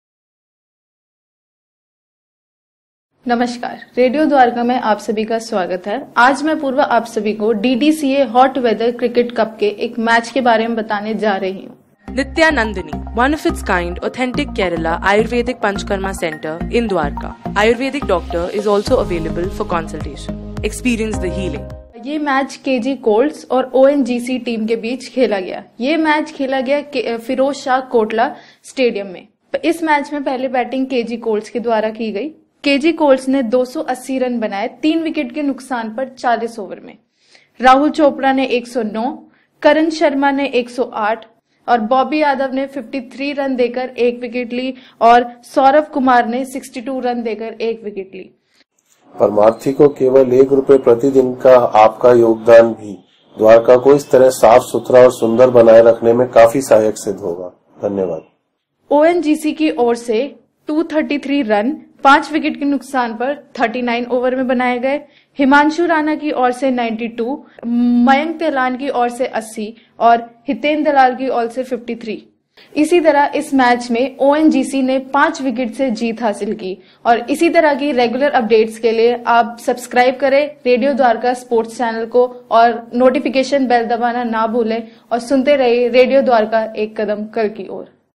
Himanshu and Mayank shine as ONGC beats KG Colts, DDCA Hot Weather cup
Half century by himanshu Rana who scored 92 and Mayank Tehlan's 80 with Hiten Dalal's 53 helped ONGC beat KG Colts by 5 wickets at Feroz Shah kotla ground.

Brief score

KG Colts - 280/3 in 40 overs. Rahul Chopra 109, Karan Sharma 108*, Bobby Yadav 1/53 and Sourav Kumar 1/62.

ONGC - 283/5 in 39.3 overs. Himanshu Rana 92 and Mayank Tehlan 80 and Hiten Dalal 53.

To Know More, Click On Picture.

Apurva,
Radio Dwarka,